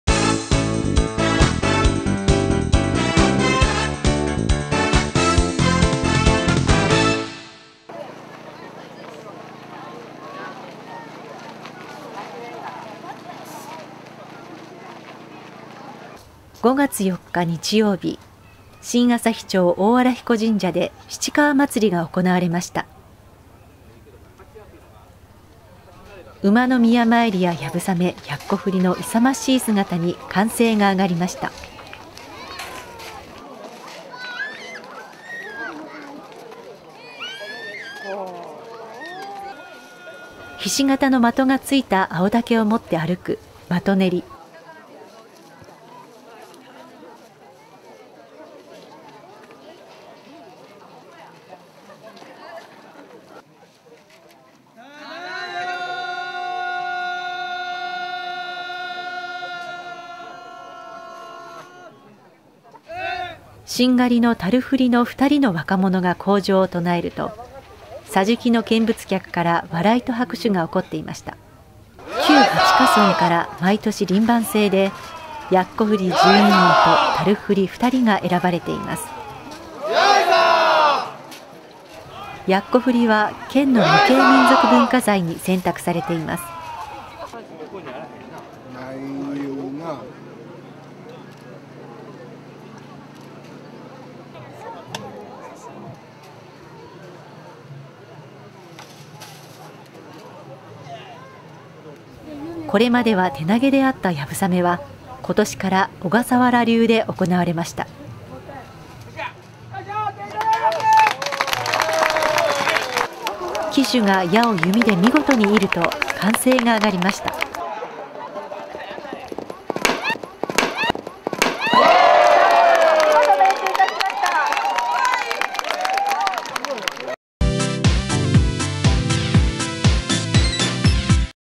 20140504七川祭り
５月４日日曜日、新旭町、大荒比古神社で「七川祭り」が行われました。
馬の宮参りや流鏑馬、奴振りの勇ましい姿に歓声が上がりました。ひし形の的がついた青竹を持って歩く「的練り」。しんがりの「樽振り」の２人の若者が、口上を唱えると、桟敷の見物客から笑いと拍手が起こっていました。旧８カ村から毎年輪番制で、奴振り12人と樽振り２人が選ばれています。やっこ振りは県の無形民俗文化財に選択されています。
これまでは手投げであった流鏑馬は、今年から小笠原流で行われました。騎手が矢を弓で見事に射ると歓声が上がりました。